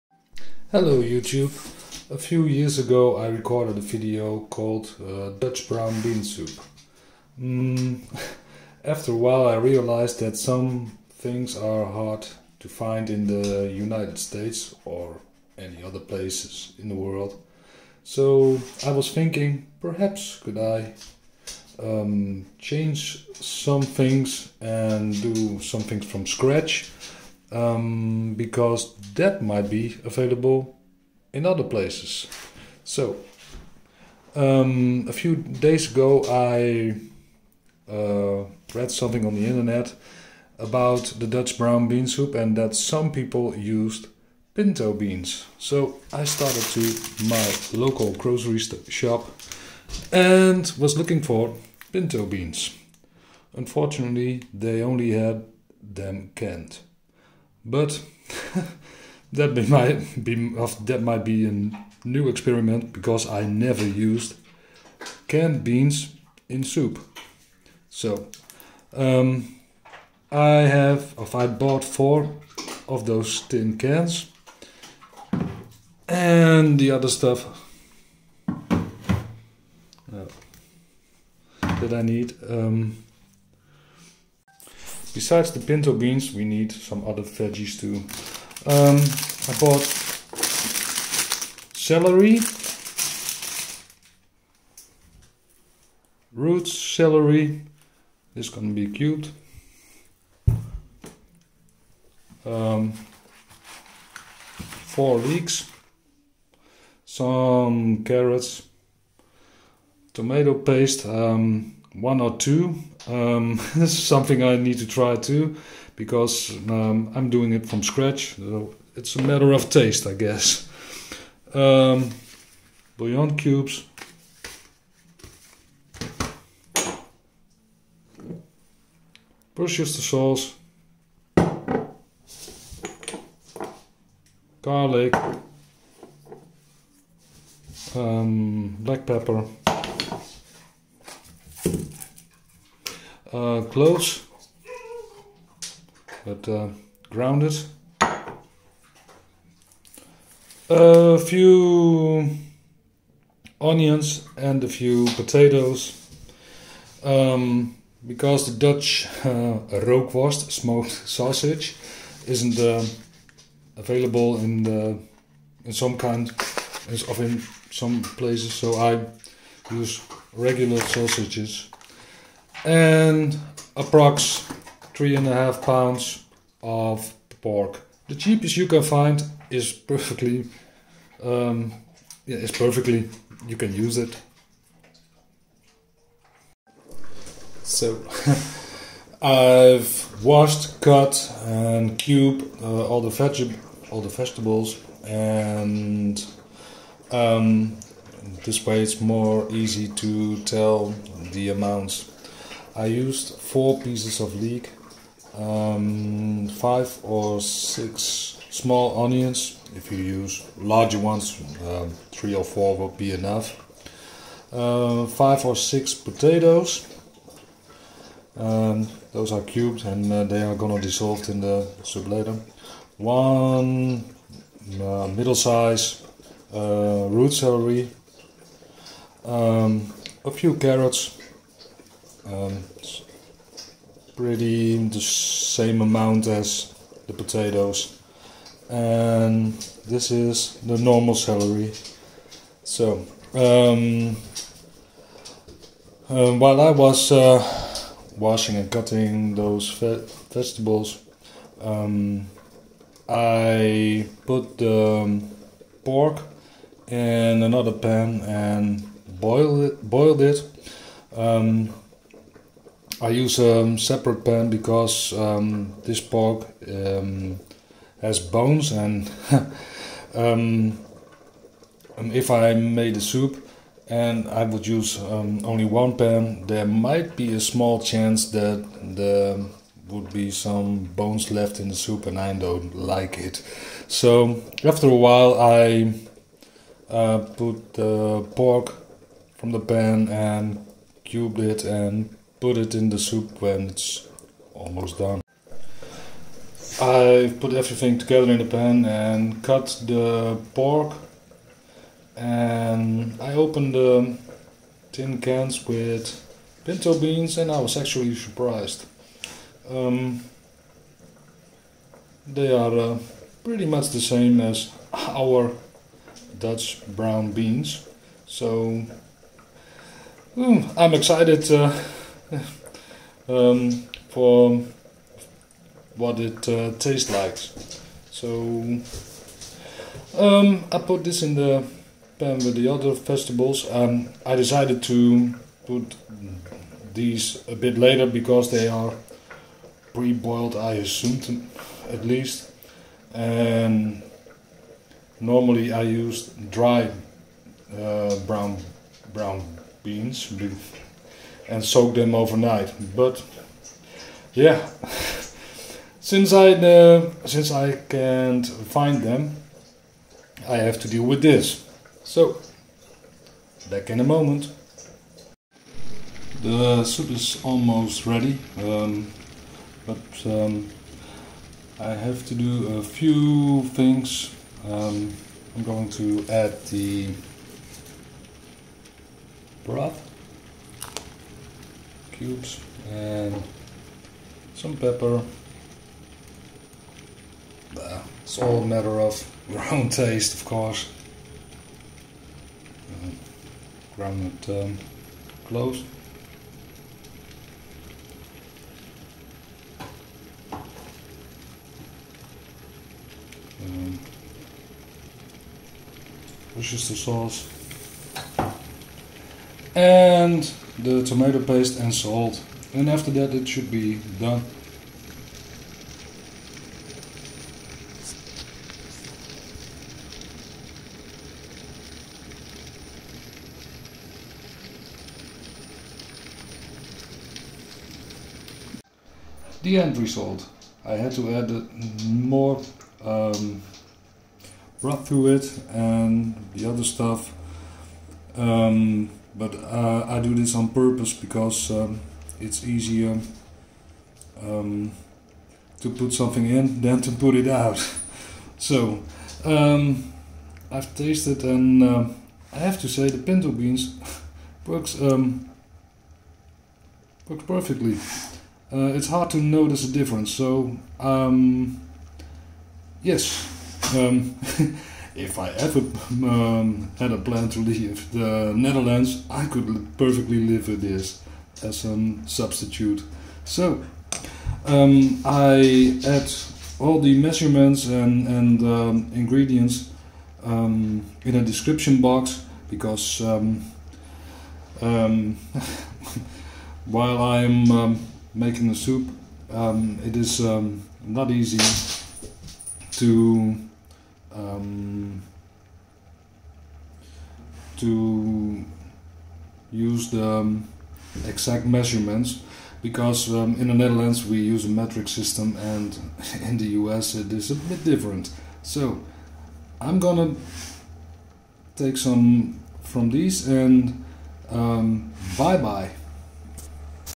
Brown Bean Soup (International Version)
Dutch Brown Bean Soup Revisited.

A few years ago I made a video about it, but there was a problem.
Some of the ingredients are not easy to obtain, so I decided to make it with more common available things.

The recipe is for a large pan, because it takes a while to make it and it is very common to put something in the freezer.

Ingredients:
4 cans of Pinto Beans or 4lbs dryed ones (ofcourse they need to soak overnight)
4 pieces of leek
3 or 4 great onions
1 root celery (medium size)
5 or 6 medium size carrots
5 or 6 medium size potatoes
some celery

2 lbs of sausages
3.5 till 4 lbs of porkchops (ofcourse the cheapest you can find)

2 small cans of tomatopaste
5 beef broth tablets

These things have to be added at your own taste
salt
black pepper
garlic powder
grounded cloves

worcestershire sauce

Good Luck!